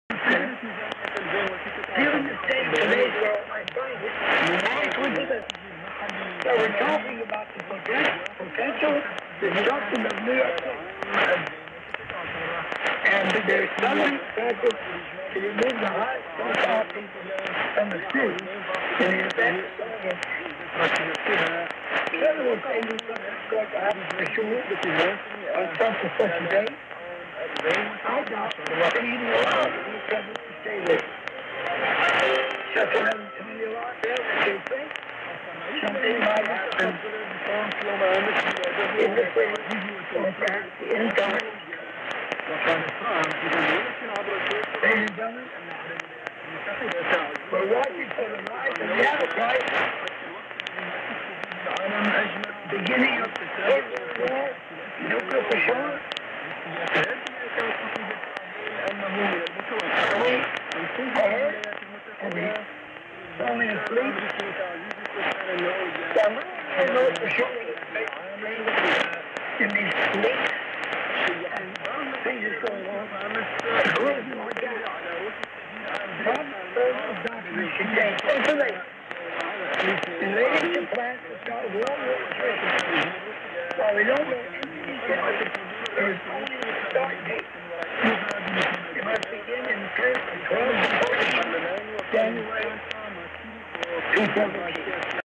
THE OVERCOMER MINISTRY VS CRI ON 5985 KHZ (24 DEC. 2016 05.10 UTC)
Local Time: UTC +13H
Frequency: 5985 kHz
Location: New Zealand, Hira
Ant.: LW more or less 80 metres
RX: Sangean ATS 909-X